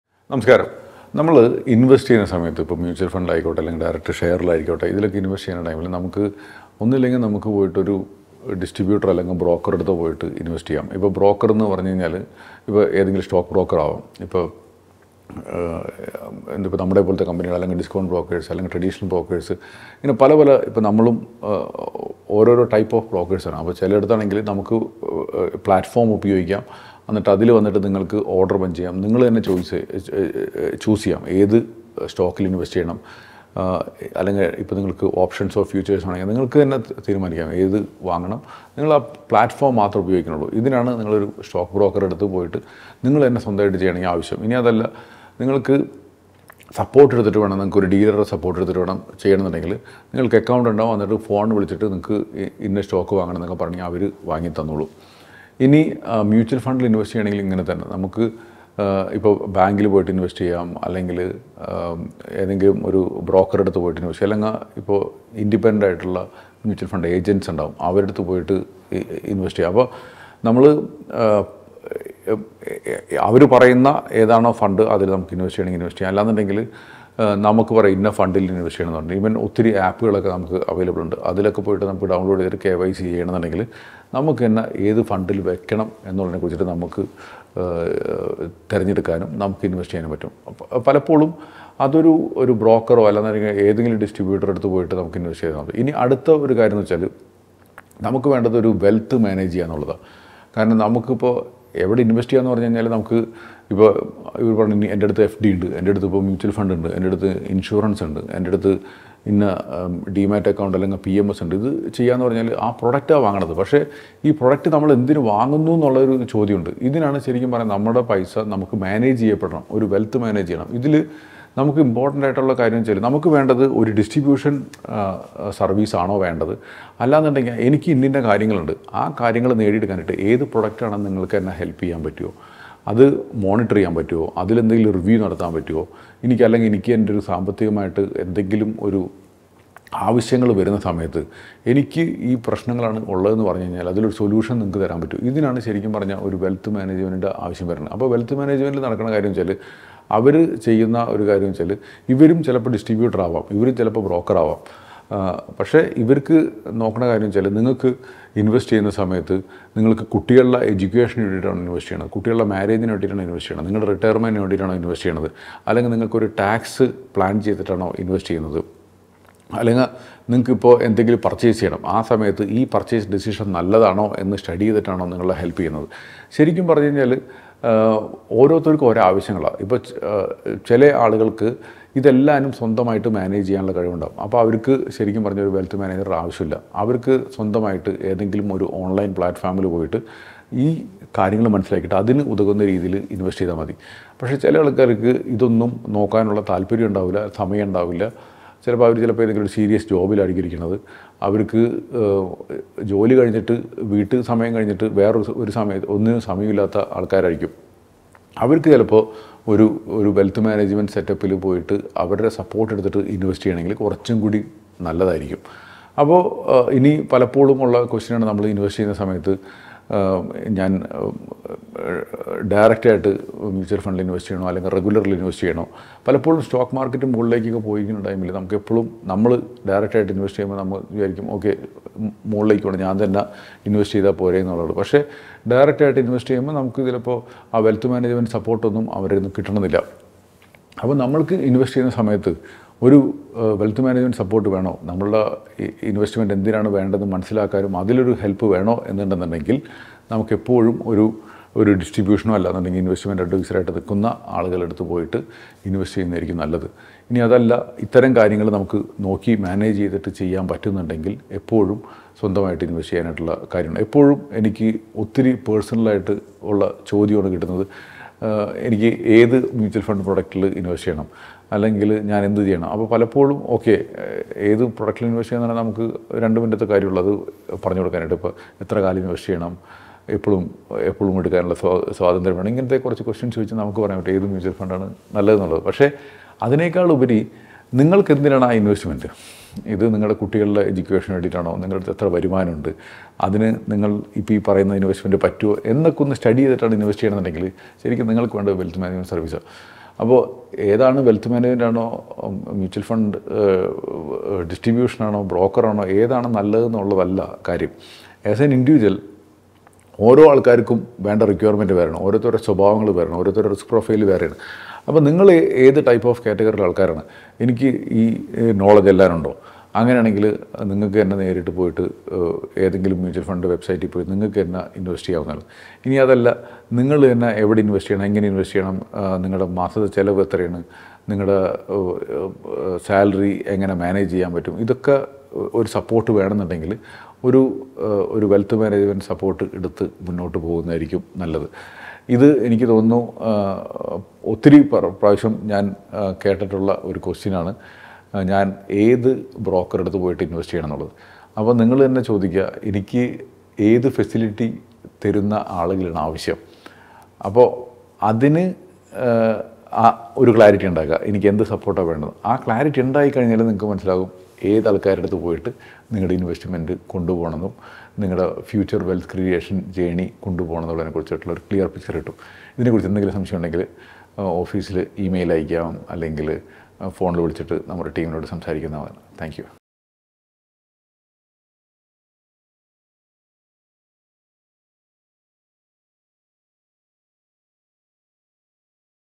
നിങ്ങൾ ഇൻവെസ്റ്റ് ചെയ്യുമ്പോൾ ഒരു വെൽത്ത് മാനേജ്മെൻറ് സപ്പോർട്ട് വേണമോ അതോ ഒരു പ്ലാറ്റ്ഫോം മതിയോ?
Thinking about investing but unsure where to start? In this video, we break down the key things every investor needs to understand before taking the plunge. From managing risks to knowing your investment options, these insights will help you make informed decisions and avoid common pitfalls.